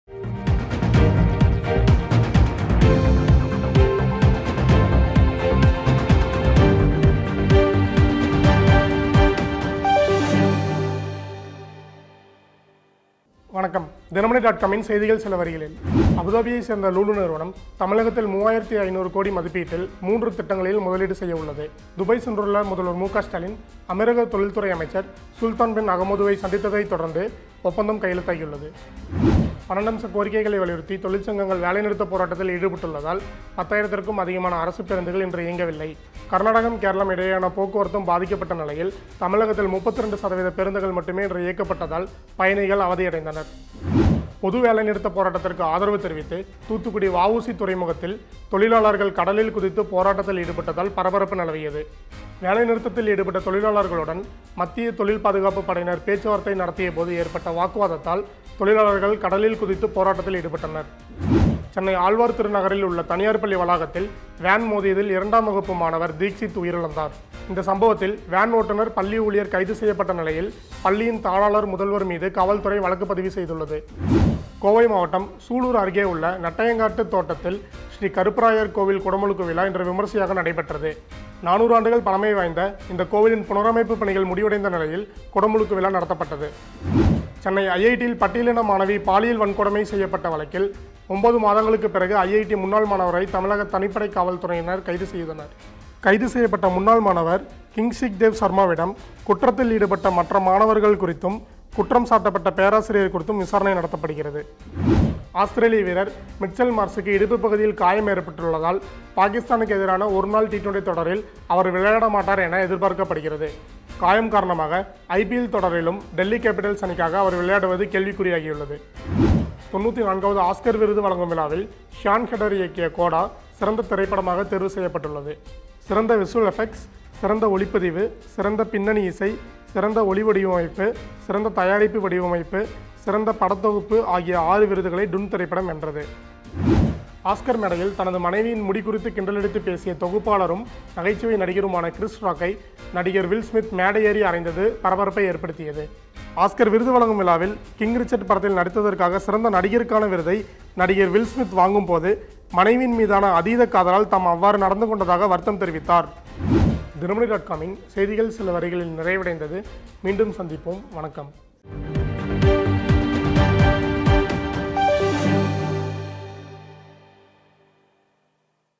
68% பேருந்துகள் இயங்கவில்லை: மக்கள் அவதி | செய்திகள்: சில வரிகளில்.. | 28.3.2022
Dinamani Daily Seithigal Morning:
Dinamani Daily Seithigal Evening:
Dinamani Daily Breaking News:
Hourly Headlines: